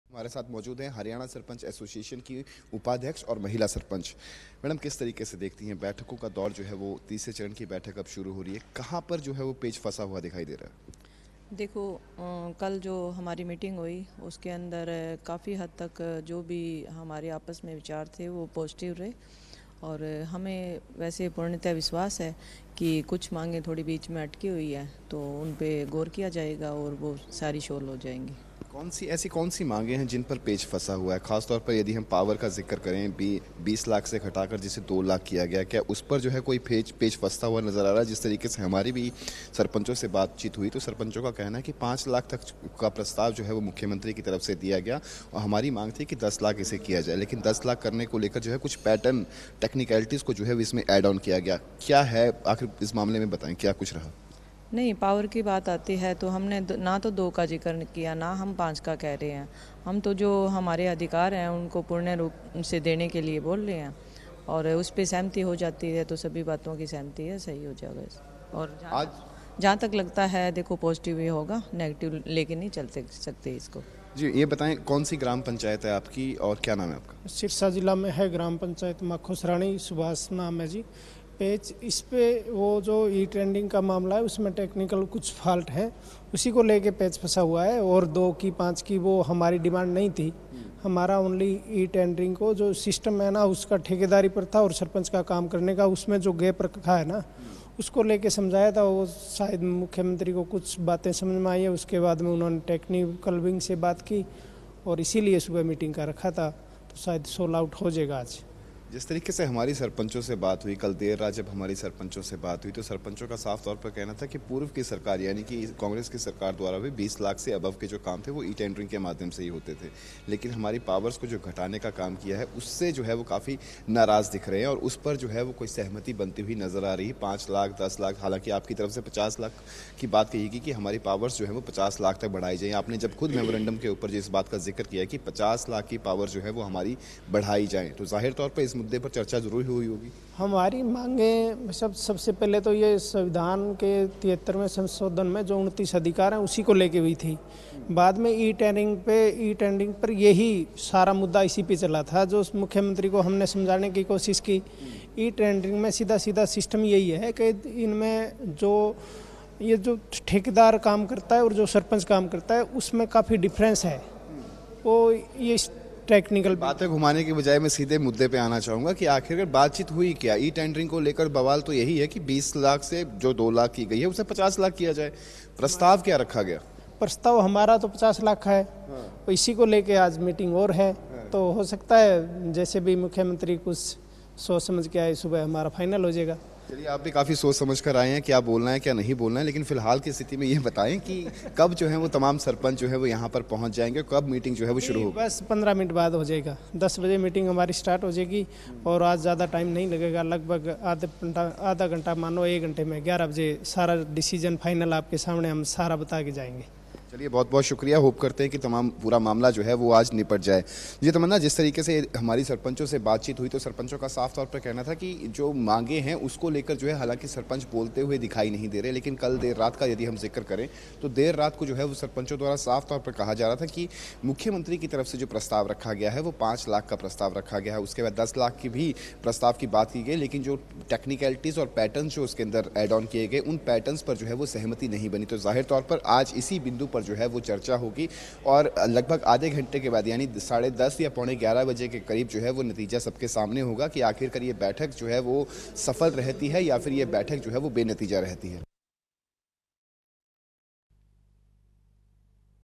तीसरे दौर की बैठक से पहले सरपंचों से खास बातचीत
अब से कुछ देर पहले हरियाणा सरपंच एसोसिएशन के प्रतिनिधियों से सूर्या समाचार ने खास बातचीत की है